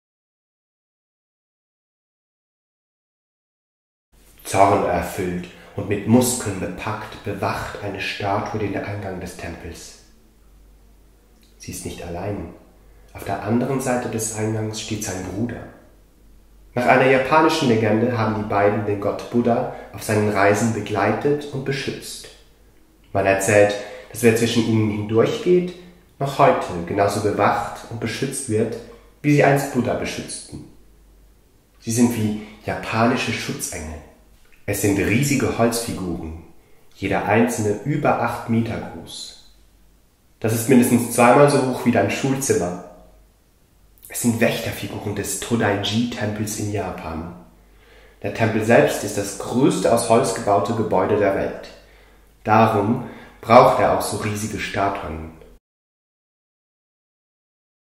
12. Wächterstatue Niō (仁王, japanisch), 1203, Todaiji-Tempel, Nara, Japan
12. Wächterstatue Niō (仁王, japanisch), 1203, Todaiji-Tempel, Nara, Japan

Zwei große Beschützer
Zornerfüllt und mit Muskeln bepackt bewacht eine Statue den Eingang des Tempels. Sie ist nicht allein, auf der anderen Seite des Eingangs steht sein „Bruder“. Nach einer japanischen Legende haben die beiden den Gott Buddha auf seinen Reisen begleitet und beschützt. Man erzählt, dass wer zwischen ihnen hindurch geht, noch heute genauso bewacht und beschützt wird, wie sie einst Buddha beschützten. Sie sind wie japanische Schutzengel.

Das größte Holzgebäude
Es sind riesige Holzfiguren, jeder einzelne über 8 Meter groß. Das ist mindestens zweimal so hoch wie Dein Schulzimmer! Es sind Wächterfiguren des Todaiji-Tempels in Japan. Der Tempel selbst ist das größte aus Holz gebaute Gebäude der Welt. Darum braucht er auch so riesige Statuen!